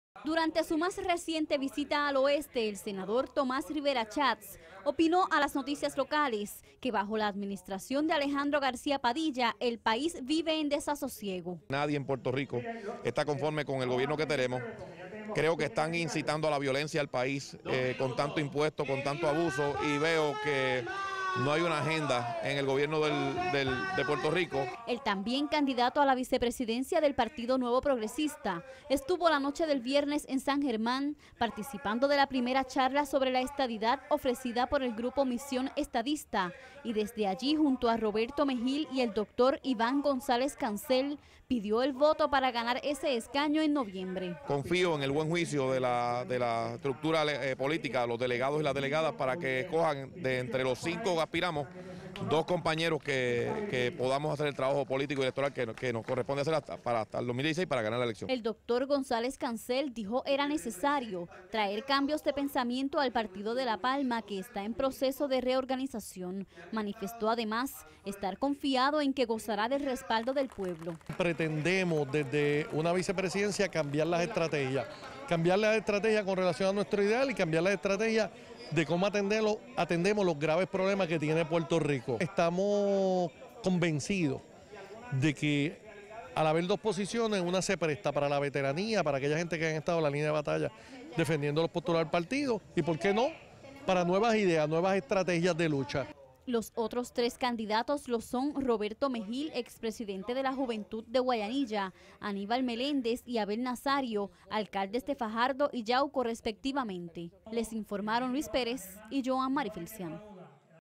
Senador PNP de Campaña por el Oeste
El Senador Thomas Rivera Schatz opinó que el gobierno popular está incitando a la violencia con la imposición de un sin número de impuestos a los comerciantes así como la falta de acuerdos para mejorar el sistema de retiro de los maestros.  Rivera Schatz estuvo de visita en el oeste como parte de su campaña a la vicepresidencia del Partido Nuevo Progresista.